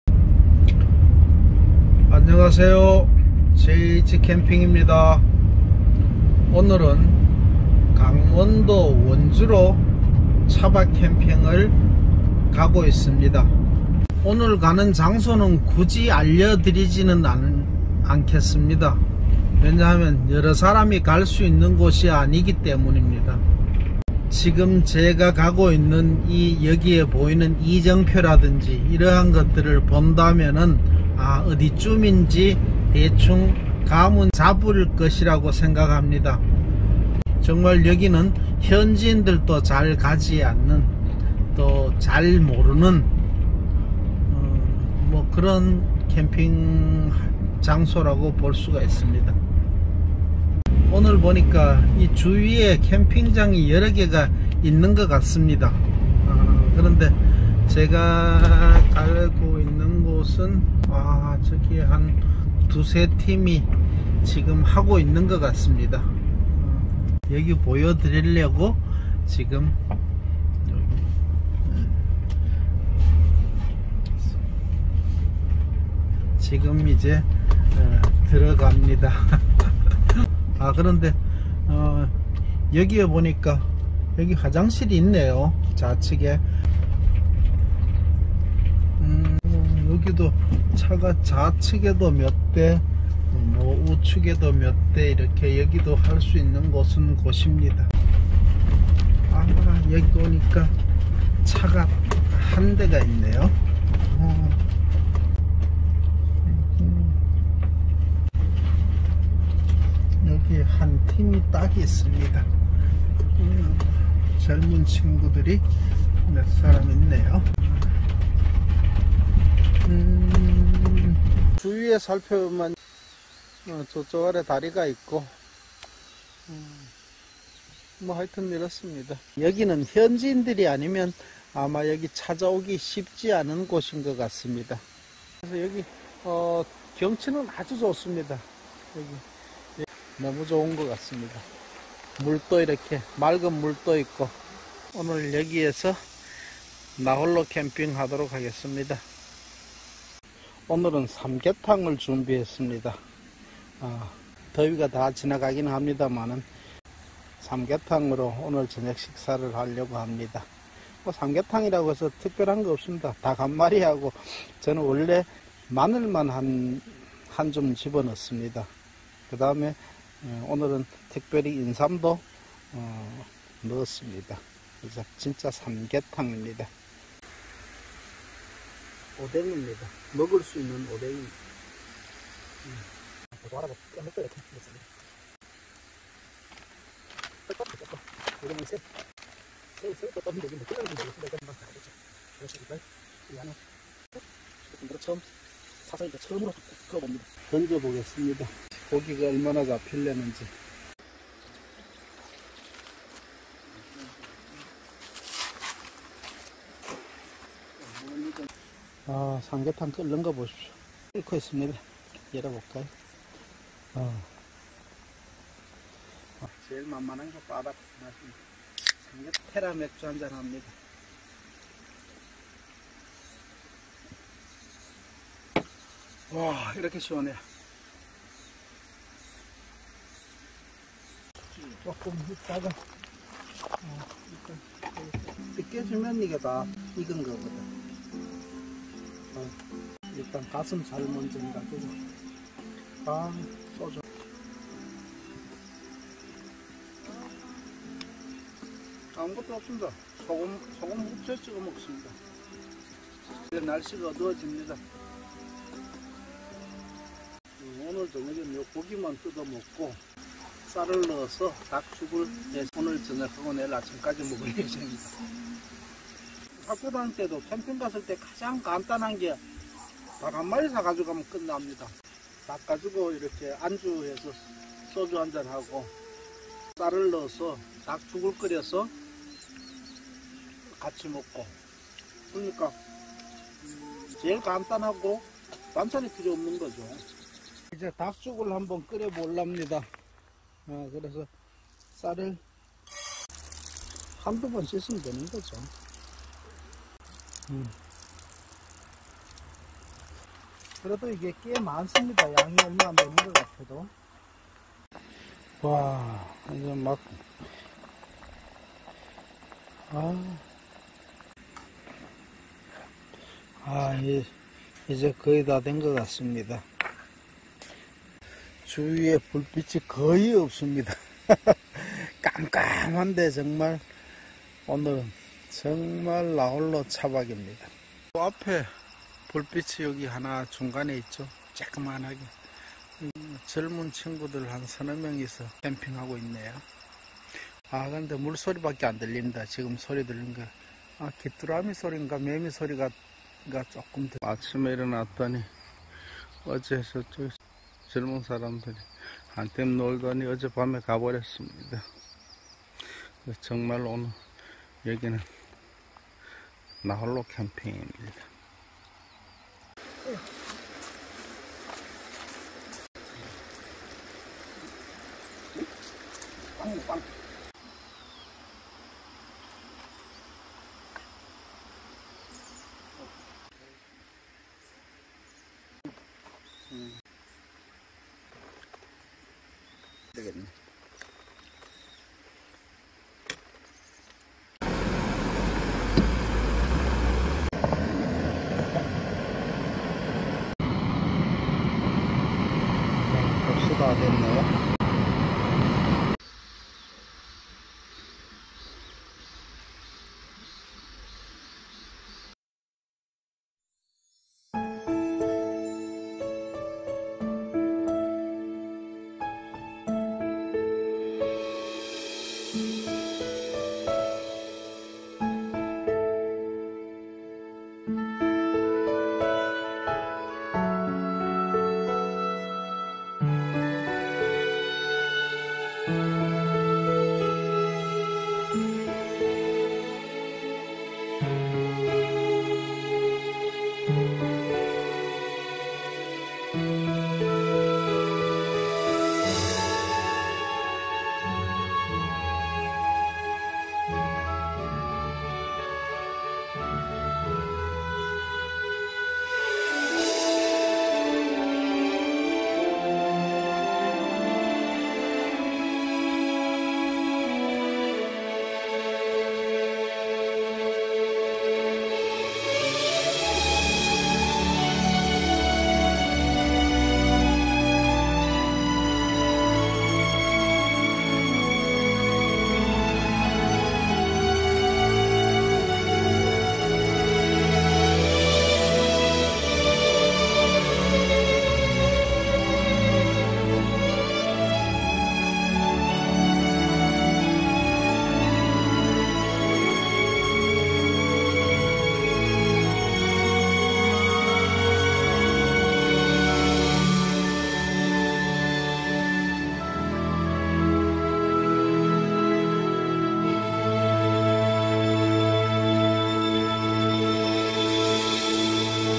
[ENG] 현지인들만 아는 판대리 노지무료 차박캠핑 / Free car park camping that only locals know
원주에  있는 현지인들만 아는 판대리 노지무료 차박캠핑